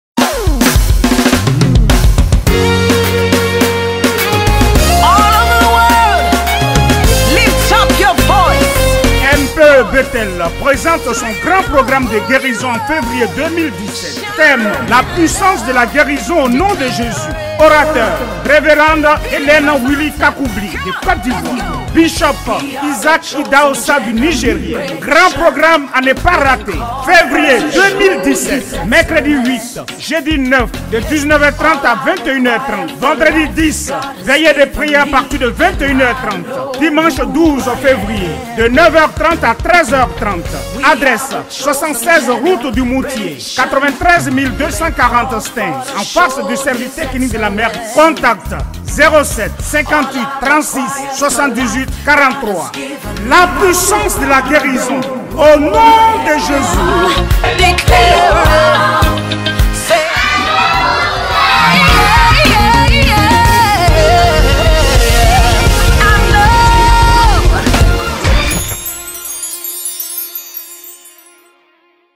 La puissance de la guérison au nom de Jesus/ Rev HELENE KACOU / Bishop Isaac IDAOSSA ( Nigeria)
MPE BETHEL Présente son Grand Programme de guérison au nom de Jésus en février 2017
Mercredi 8 février de 19h 30 à 21h 30
Jeudi 9 février de 19h30 à 21h30
Vendredi 10 février une veillée de Priere de 21h30 à 6h du matin
Dimanche 12 février de 9h30 à 13h30
Orateurs :
Rev HELENE WILLY KACOU BLY ( de côté d'ivoire )
Bishop Isaac IDAOSSA ( du Nigeria)
Adresse : 76 route du Moutier 93240 À STAINS
En face du service technique de la mairie

il est strictement interdit de télécharger ou vendre nos vidéos pour quelque raisons que ce soit ,même de remettre ça dans vos compte